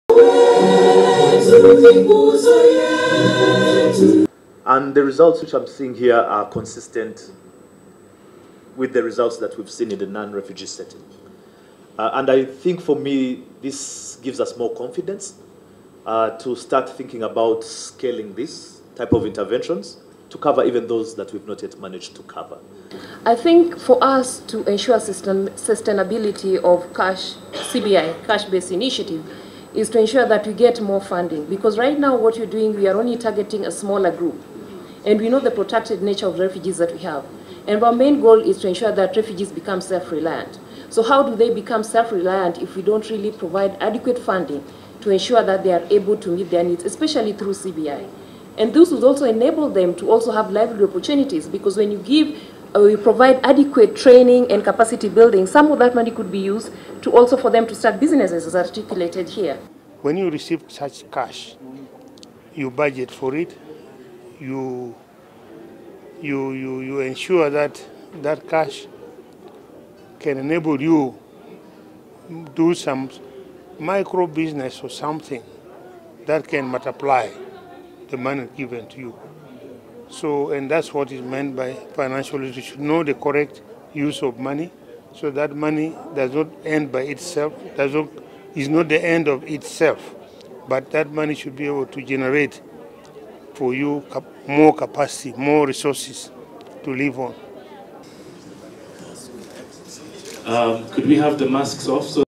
Refugee host communities need financial literacy skills - Minister Onek
The Minister for Disaster Preparedness and Refugees, Hillary Onek wants refugee communities benefiting from the one thousand dollar grant provided by Give Directly, a non-governmental organization to be equipped with financial literacy skills. The call by the minister follows a review of the performance of the grant in the host community of Kiryandongo which has seen 10,000 households now more resilient. The grant to households started in 2019.